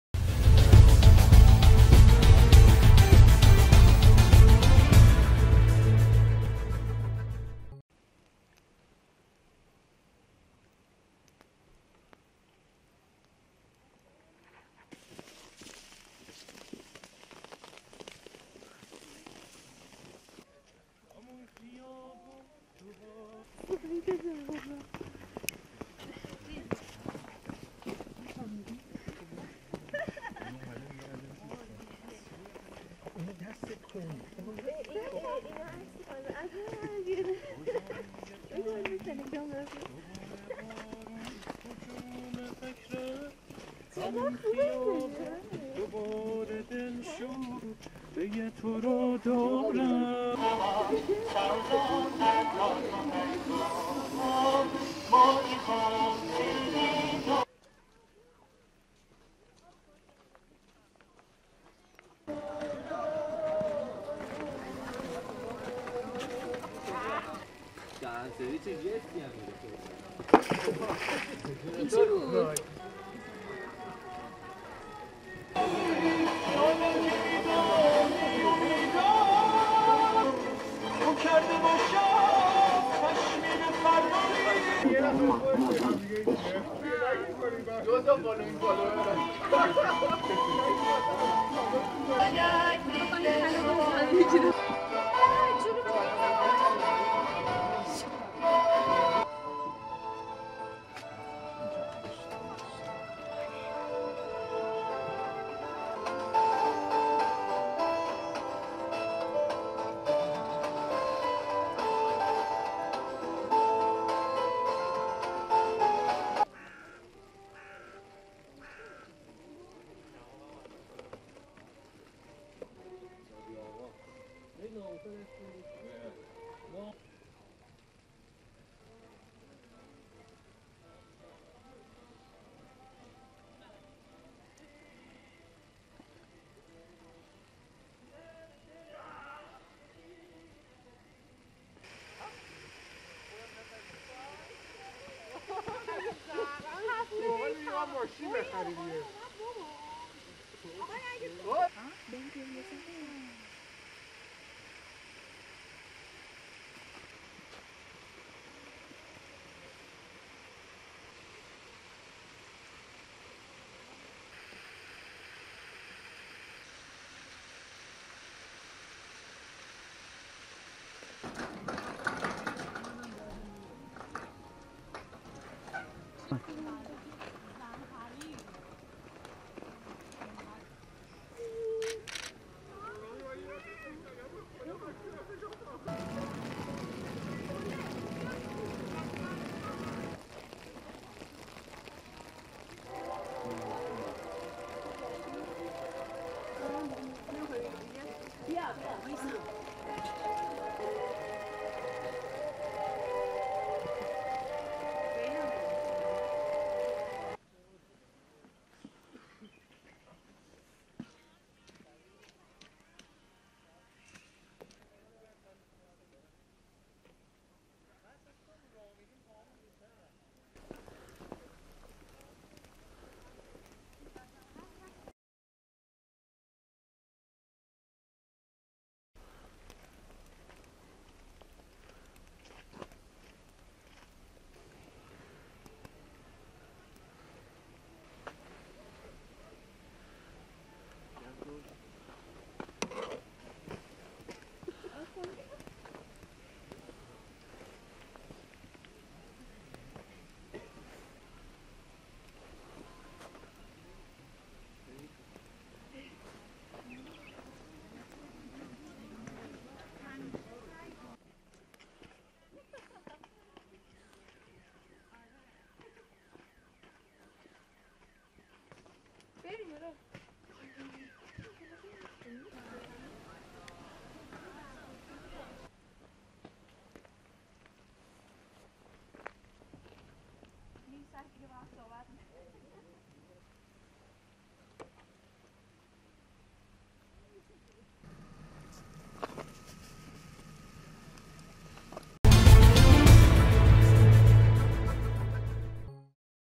مصر العربية | أول ثلوج الموسم في طهران
تنفس الايرانيون الصعداء اليوم الثلاثاء مع أول تساقط للثلوج هذا الموسم على العاصمة طهران ومدن أخرى وسط إرتفاع نسبة التلوث في الجو إلى أرقام قياسية.